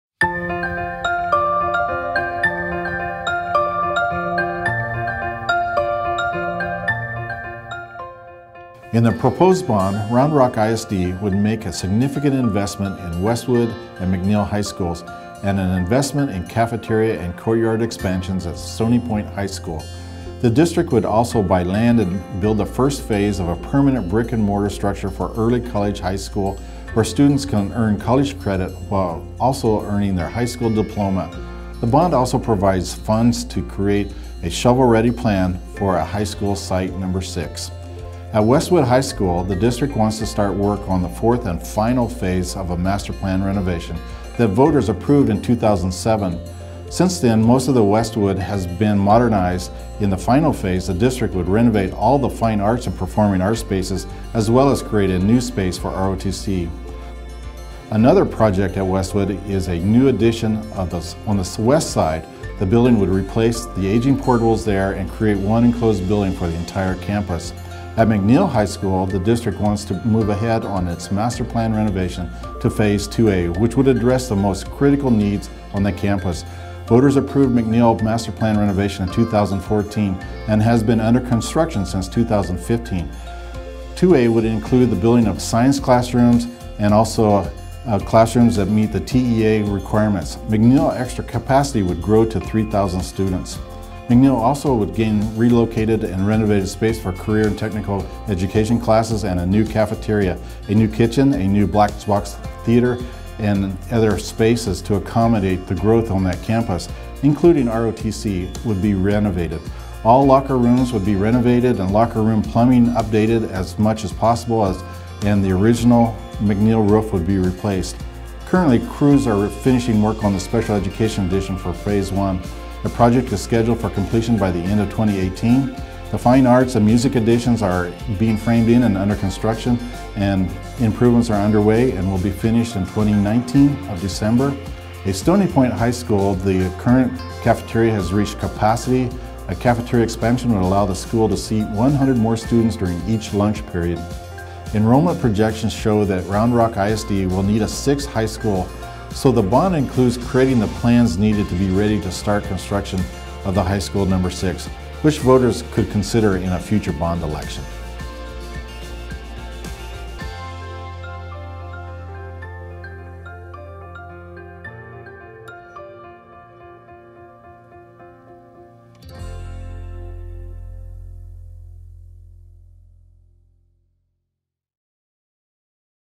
Project Impact: High School Master Plans
Bond Project Impact: Bond 2018 would continue work on high school Master Plans, address growth and security issues